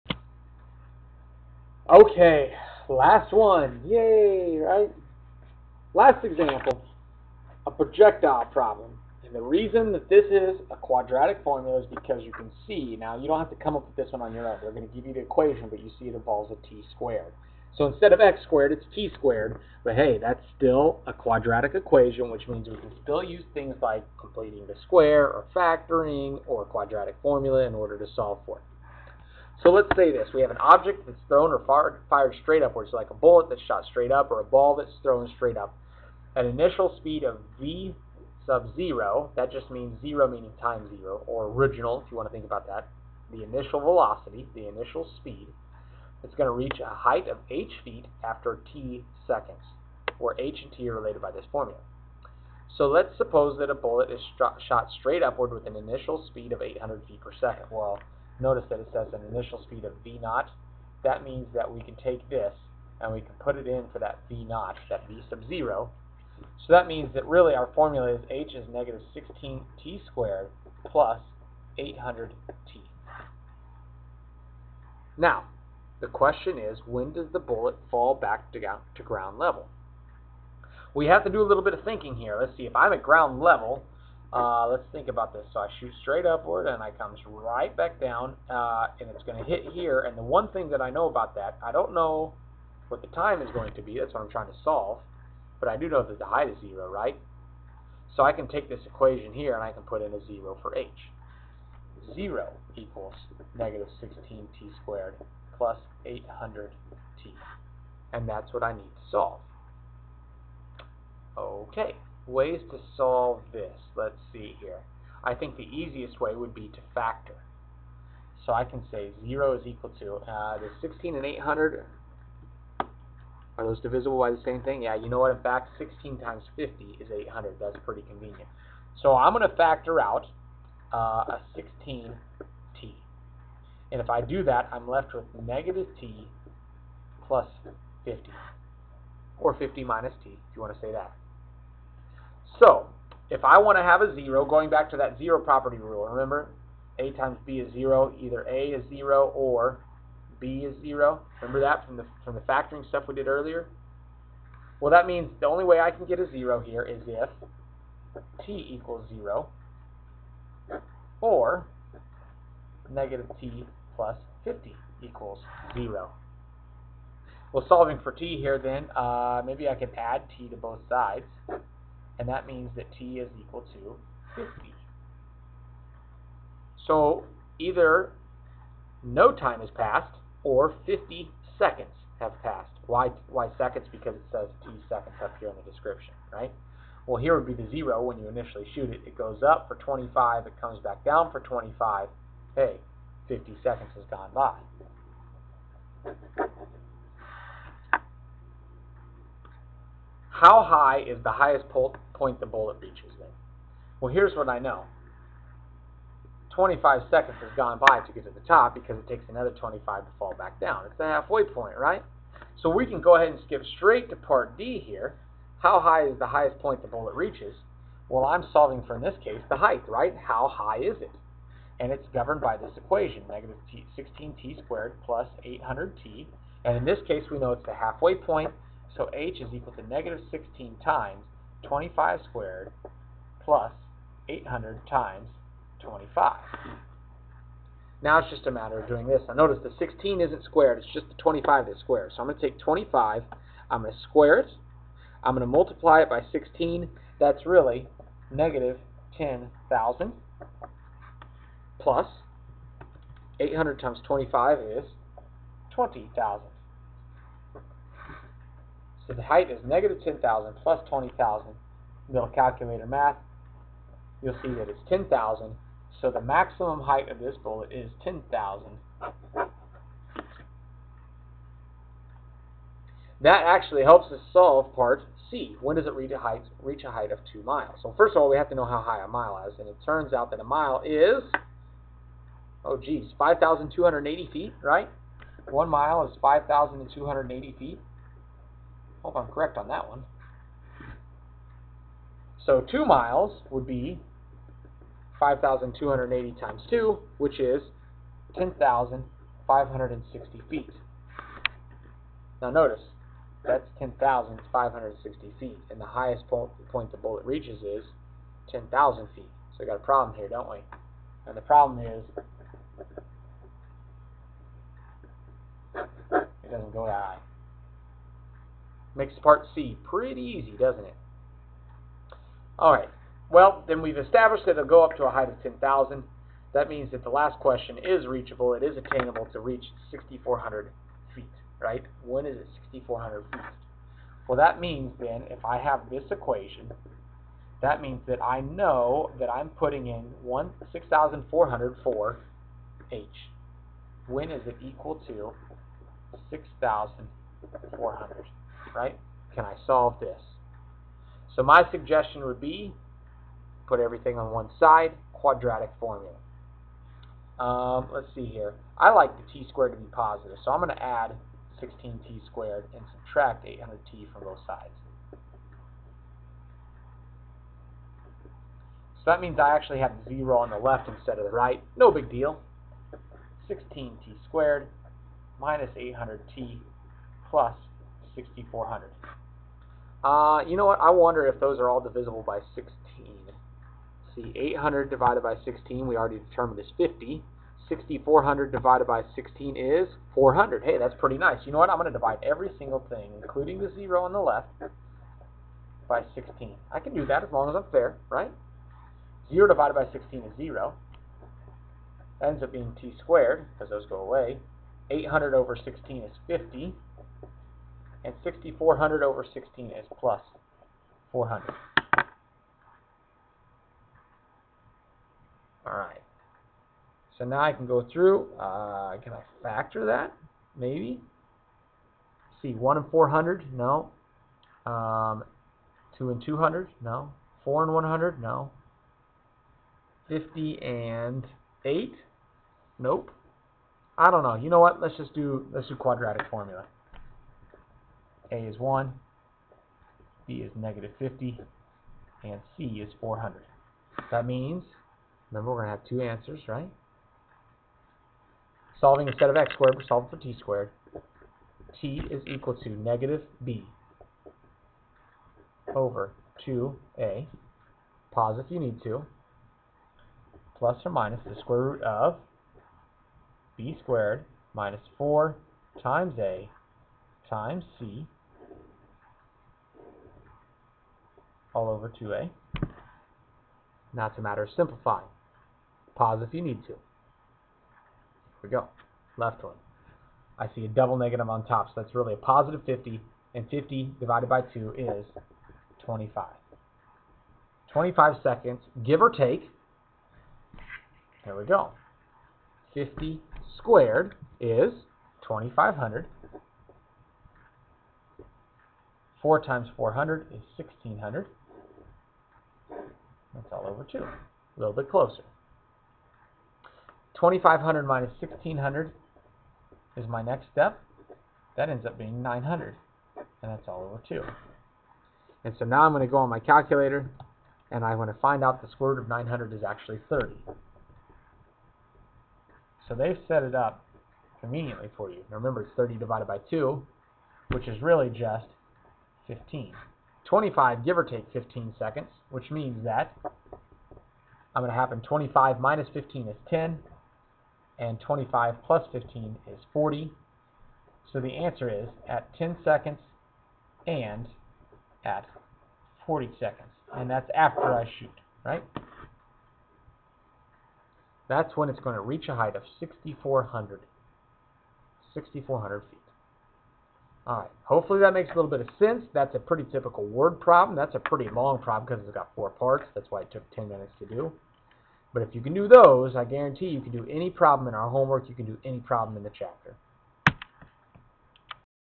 Lesson 1.3 Video 12 - Quadratic Word Problem #2 (Projectiles)
In this problem, I solve another typical word problem that involves a quadratic equation: the height of a projectile.